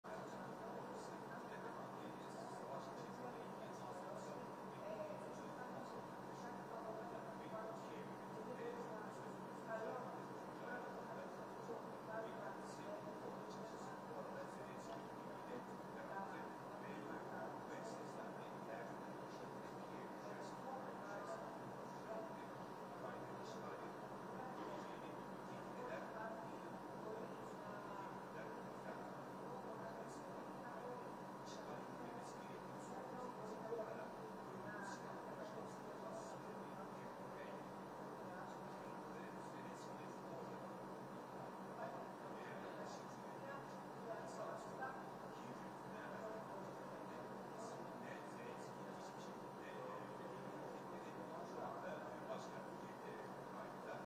Kiev cat checks safety
LIVE VIEW of Kyiv, Ukraine, after Russian invasion.